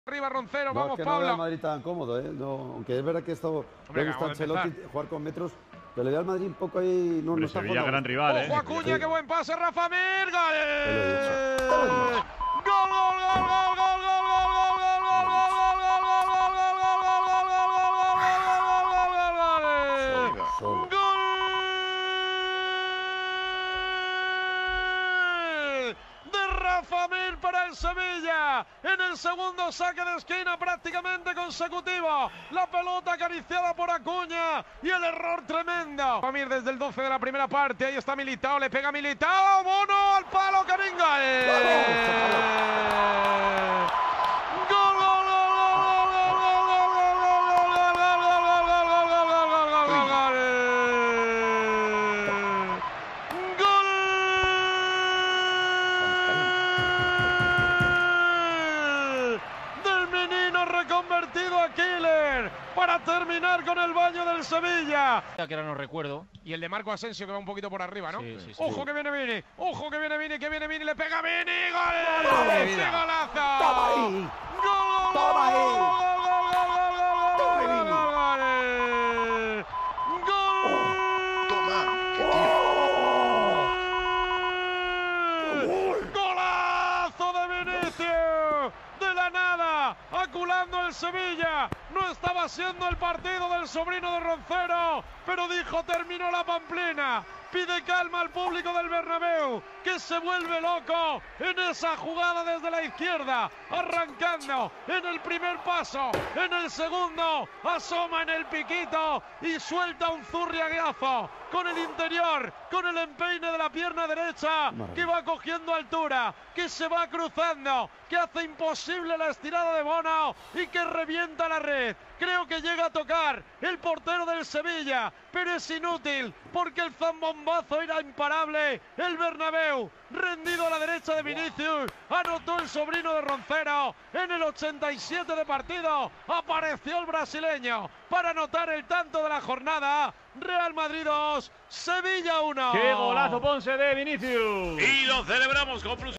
REAL MADRID 2-SEVILLA 1 | RONCERO, de la premonición a bailar con VINICIUS | AS
Tomás Roncero vivió la victoria ante el Sevilla desde la redacción de AS. Se esperaba el gol de Mir y celebró por todo lo alto el golazo de Vinicius.